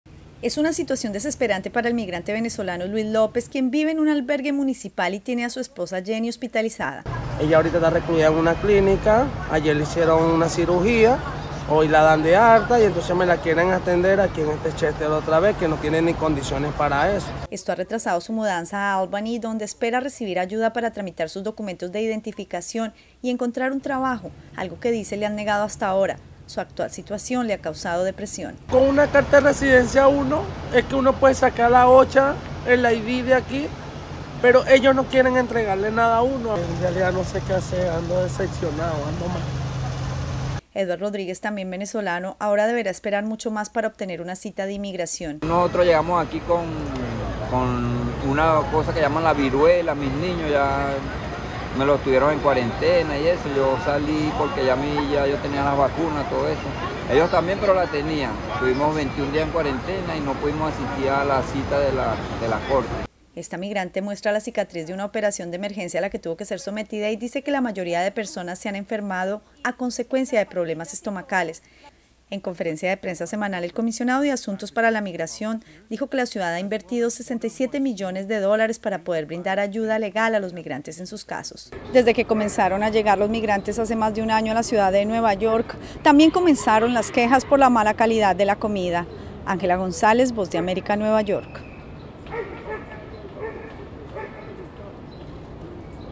Problemas de salud complican planes de migrantes en Nueva York
Migrantes que llegan enfermos o se han enfermado en los albergues de la ciudad de Nueva York no han podido cumplir con sus citas en inmigración u obtener traslados. Ángela González de la Voz de América nos trae testimonios.